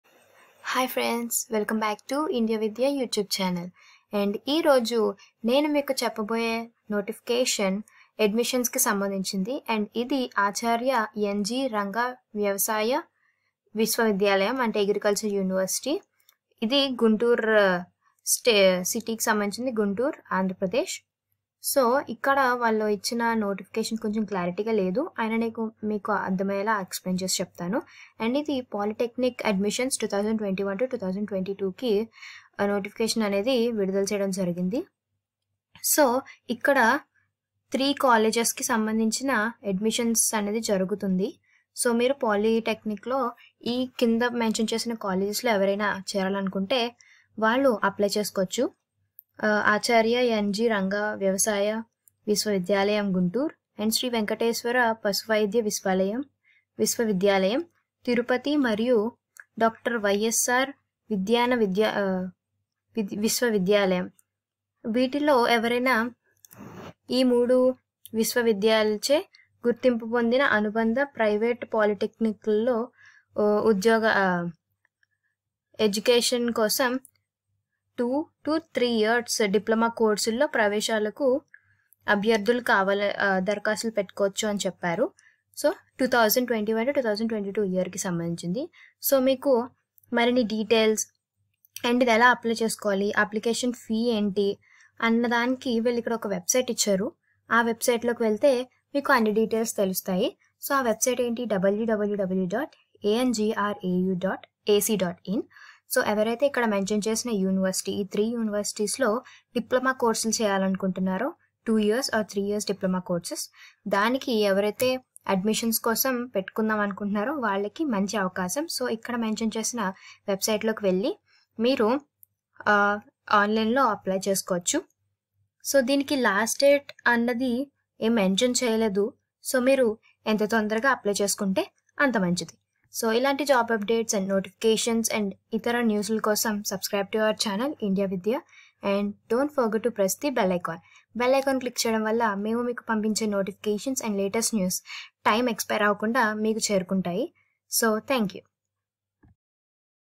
ANGRAU Guntur Polytechnic Admissions 2021-22 Diploma Courses in Agriculture and allied subjects
Acharya NG Ranga University Polytechnic Admissions 2021-22